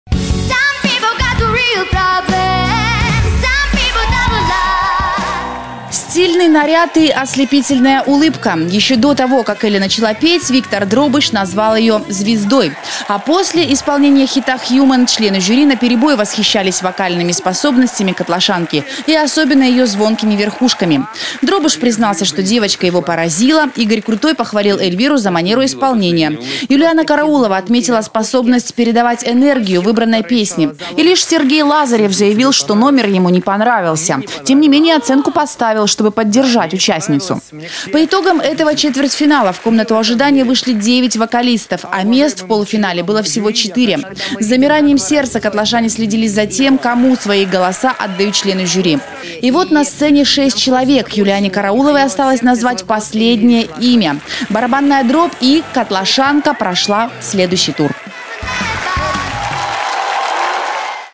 Эльвира прошла дальше
Котлашанка Эльвира Филатова прошла в финал международного детского вокального конкурса «Ты супер» на канале НТВ.